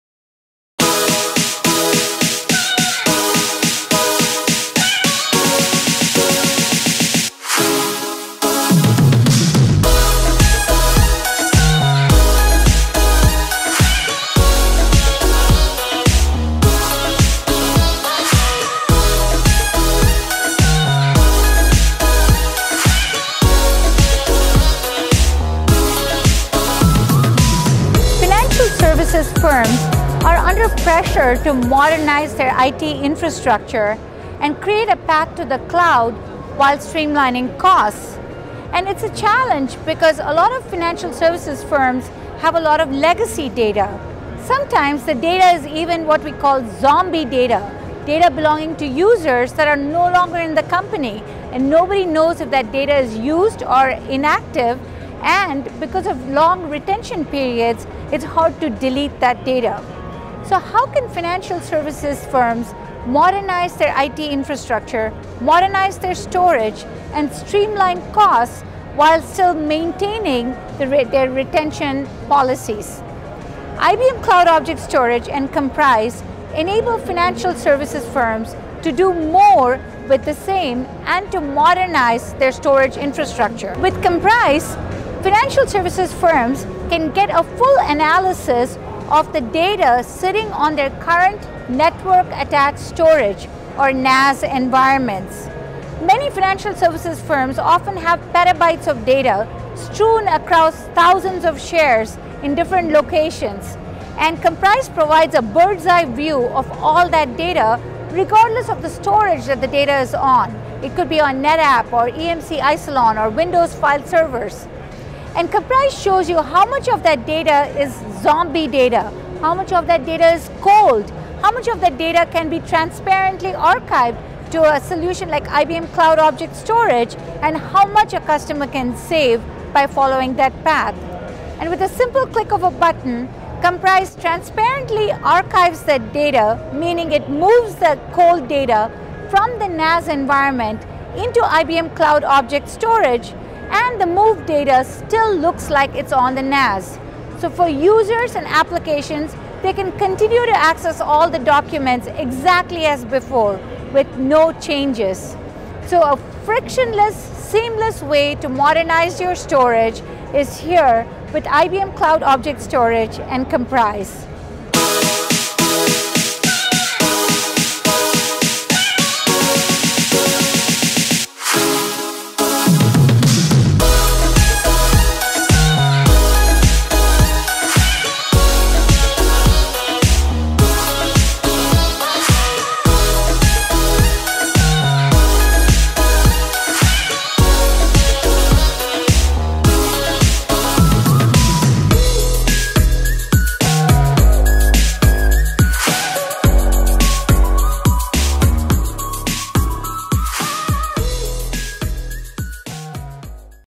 Financial Services: Cut the Costs of Storing Financial Data
As data continues to grow at an exponential rate and regulatory requirements expand, the majority of IT budgets stay flat or even shrink. Komprise provides a comprehensive solution for managing data that provides Financial Service organizations with crucial analytics and insight into their data, as well as the tools they need to lower storage costs by 70%+. The best part? Komprise does this without causing any disruption to employees or mission-critical applications.

To learn how other financial services institutes have been able to safely move data to the cloud to reduce storage costs